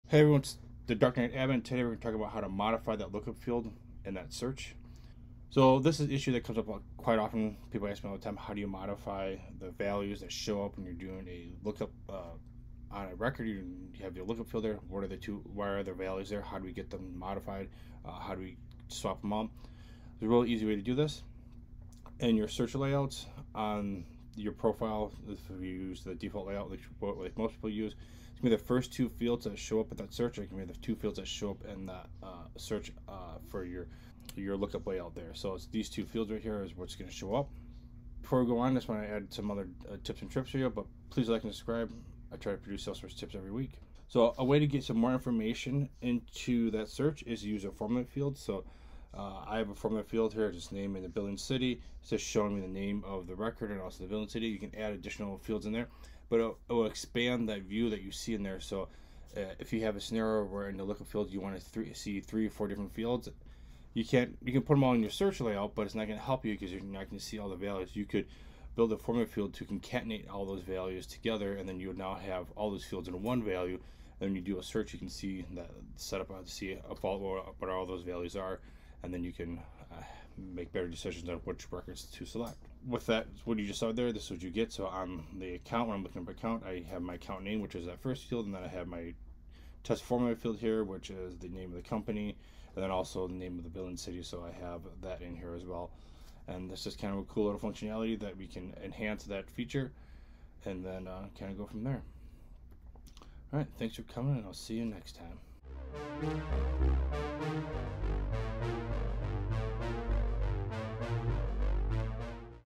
Salesforce Lookup Field Search
How to modify the fields that show up in the lookup search.
Trick to show more data in the lookup search